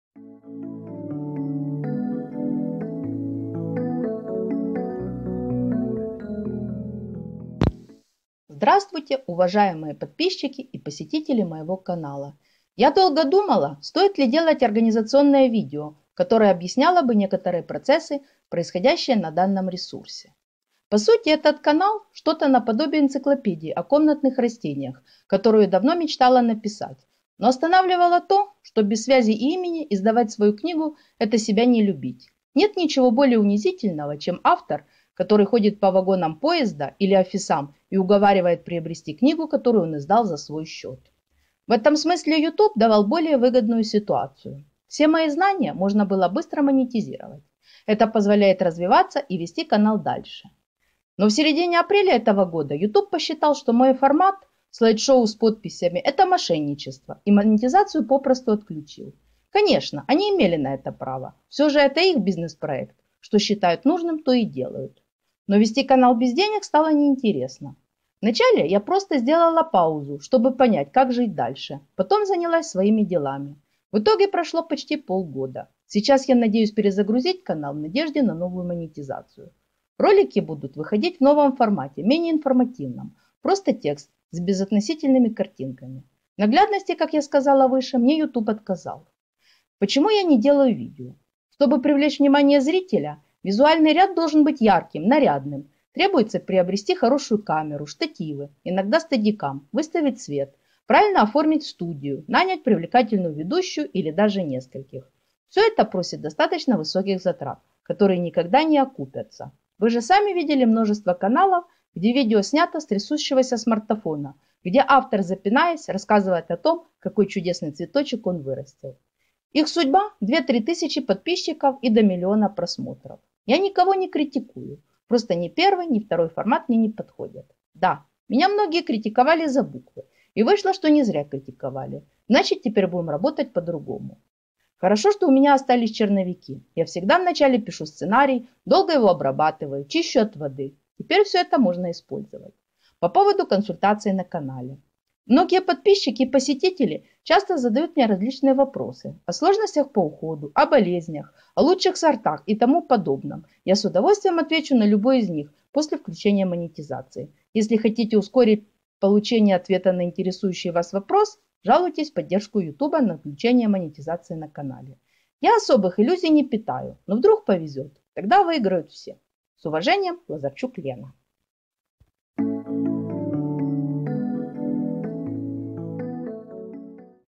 Перезагрузка канала | Отключение монетизации | Организационные вопросы
Внимание!

Я долго думала, стоит ли делать организационное видео, которое объясняло бы некоторые процессы, происходящие на данном ресурсе.